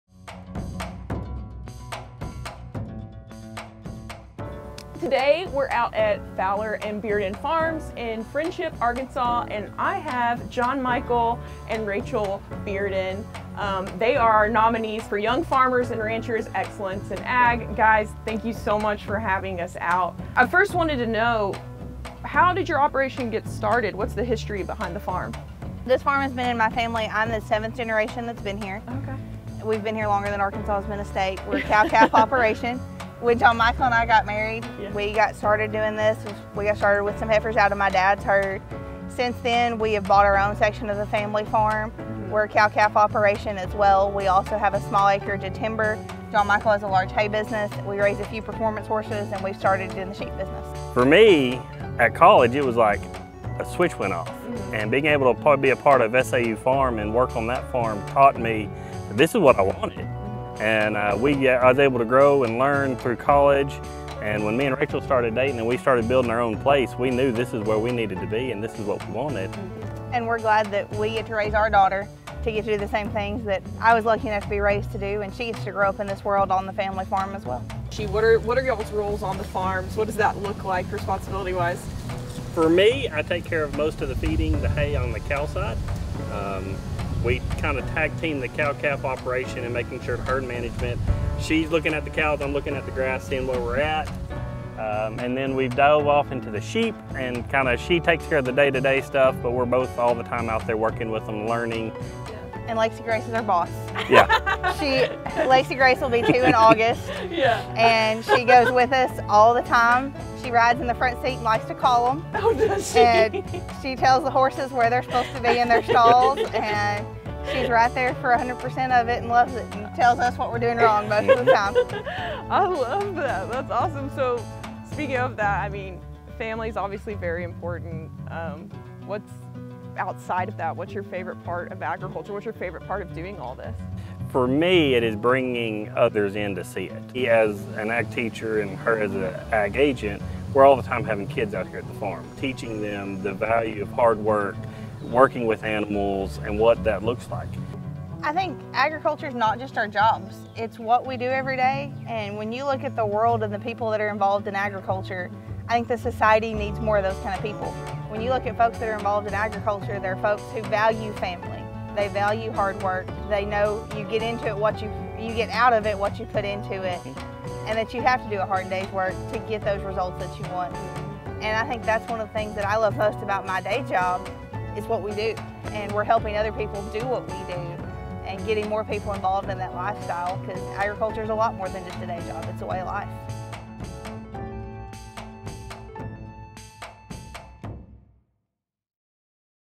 2021 YF&R Excellence in Agriculture Award Winner | The Bearden Family
The 2021 YF&R Excellence in Agriculture Award winners were John Michael and Rachel Bearden. The Beardens raise cattle, sheep, performance horses and hay along the Ouachita River. John Michael is the Malvern High School agriculture teacher and Rachel is the Hot Spring County Extension agent. They have a daughter, Lexi Grace. They received an $11,000 cash prize and a trip to the 2022 American Farm Bureau Convention to compete for the national award.